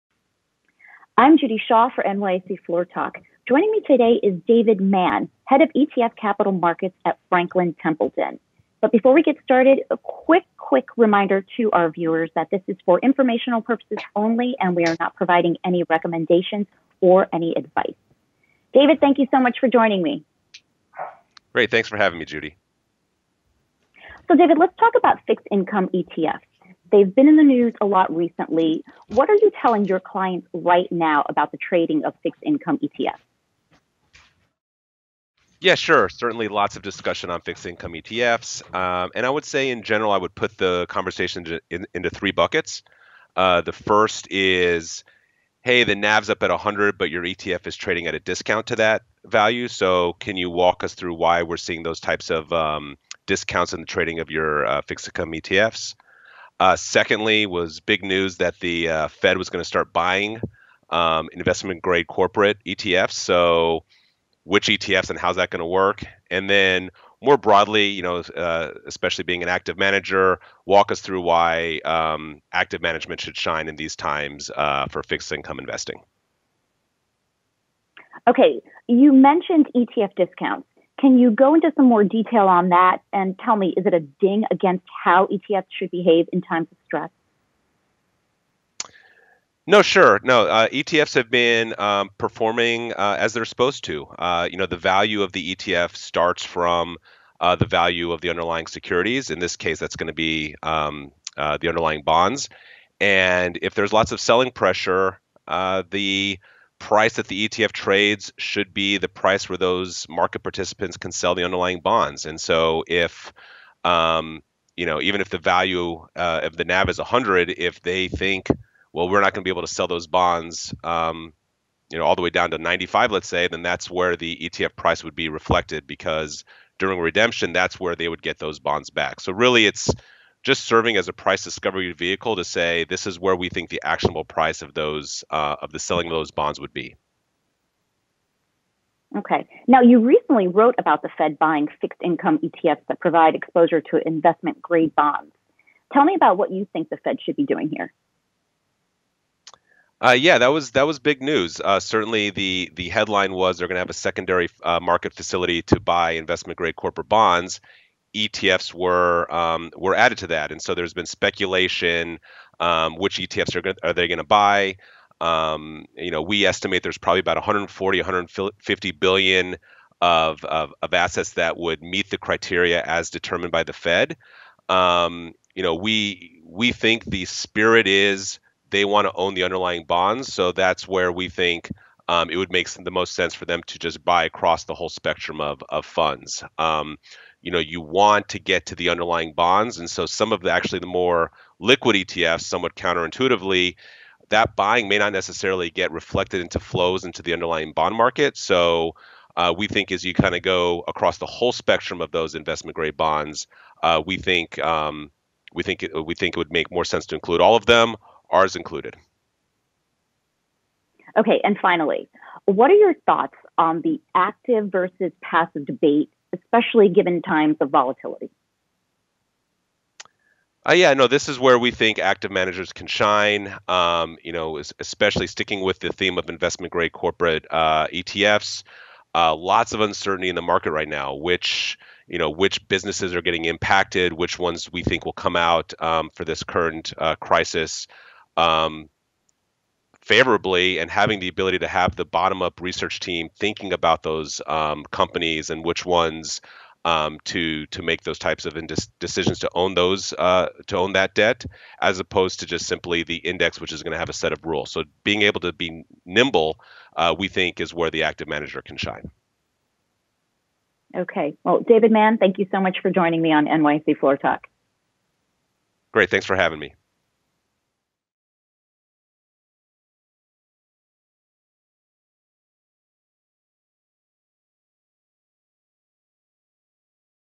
NYSE Floor talk with Franklin Templeton's David Mann
We talk to Franklin Templeton's David Mann about the Fed buying fixed income ETFs for exposure to investment grade bonds NYSEFloortalk.